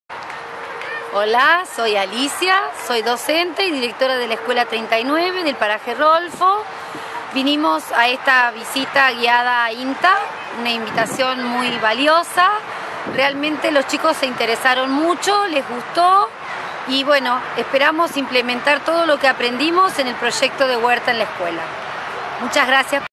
Alicia en Vidriera Tecnológica en 2015
La docente de la Escuela 39 Paraje Rolfo cuenta su experiencia con los alumnos en la Vidriera Tecnológica